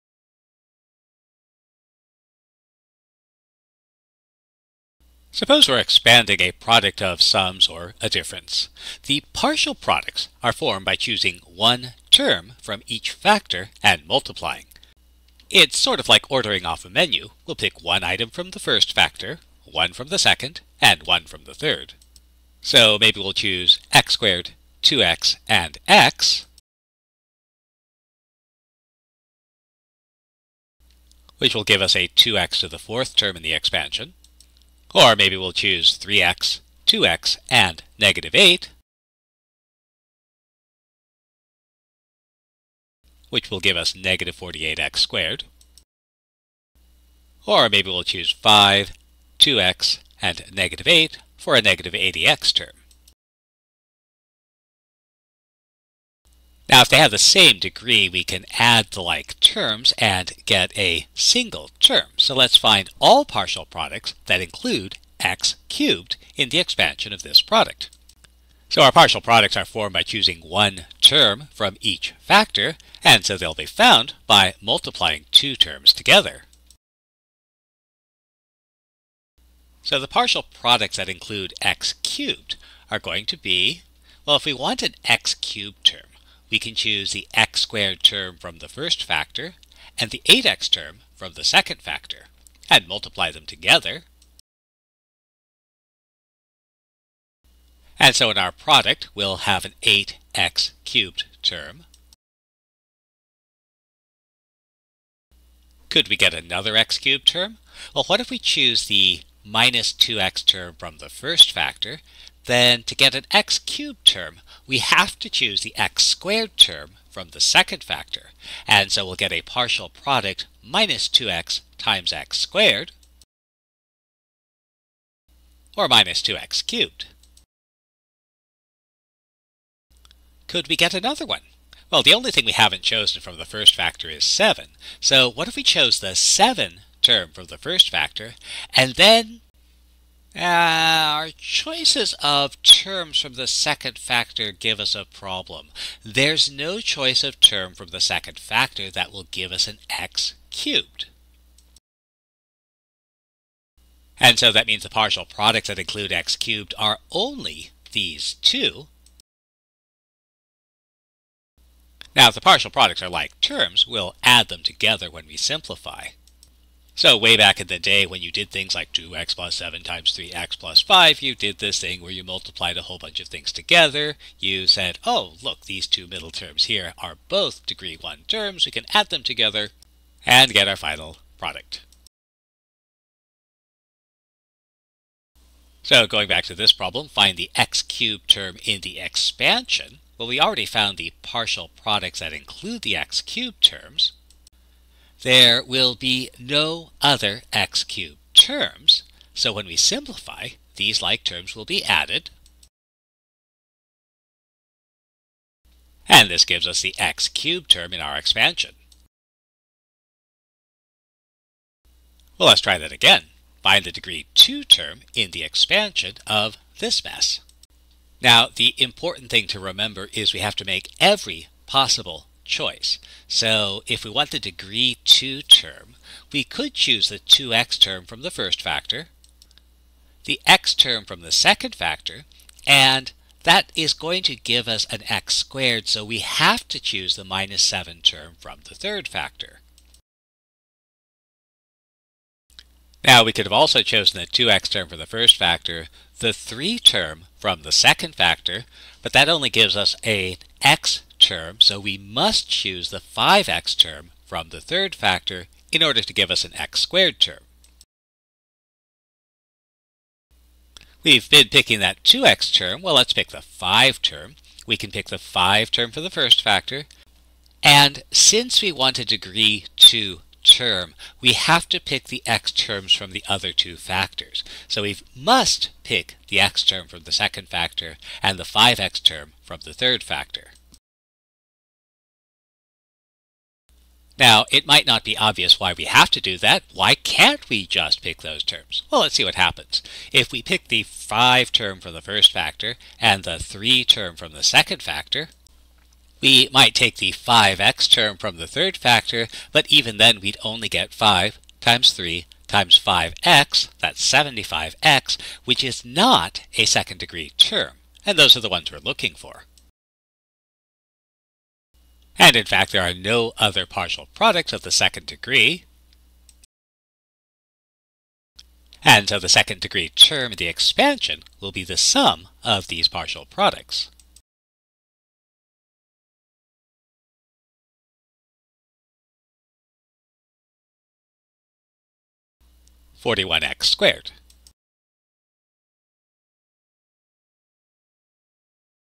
Binomial Theorem, Part One
Introduction to the expansion of polynomials using the binomial theorem.